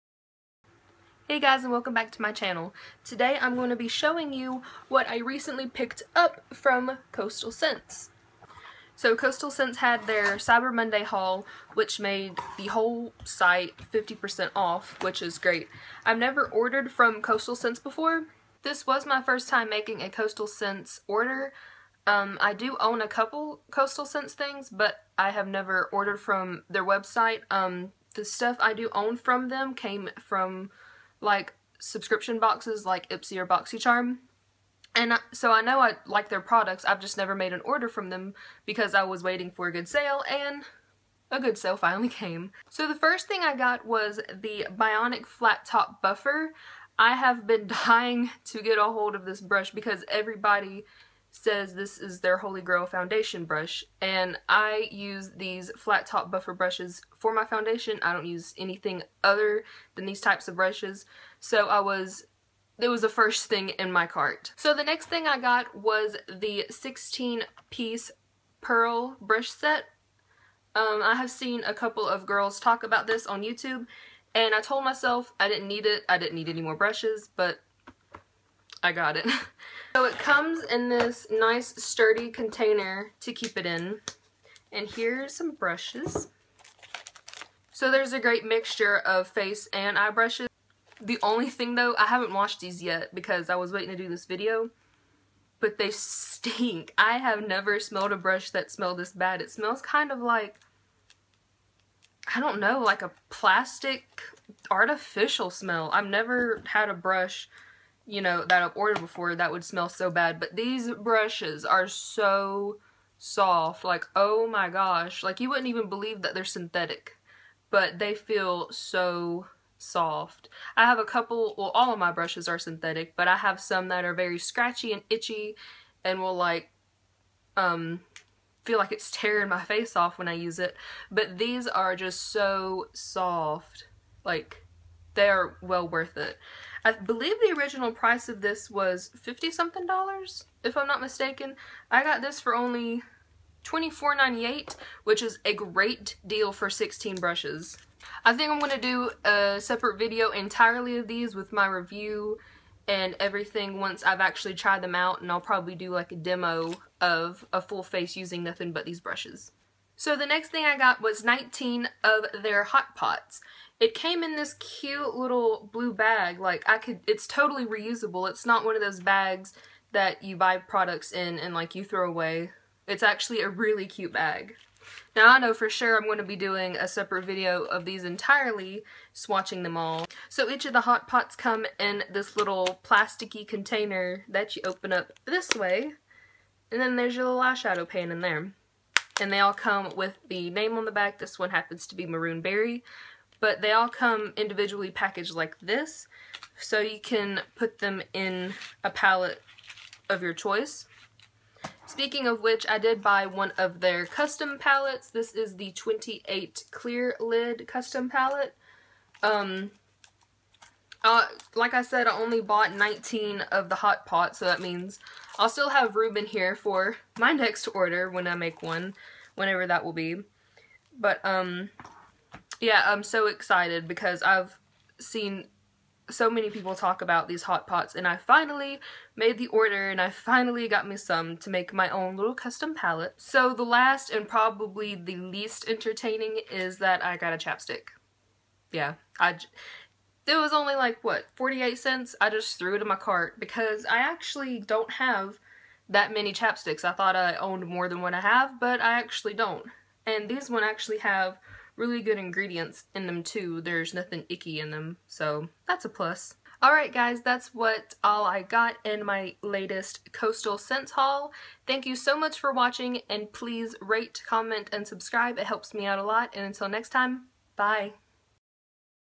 My First Ever Coastal Scents Unboxing | Cyber Monday Haul
Here’s a quick video of what I got in my Coastal Scents haul!

Products shown:
Bionic Flat Top Buffer
16 Piece Pearl Brush Set
Whole lotta Hot Pots! :D
28 Piece Custom Palette with Clear Lid
Lip Affection in “Charming Cherry” (lip balm)

Lipstick I was wearing: Milani Color Statement Lipstick in “Rose Hip”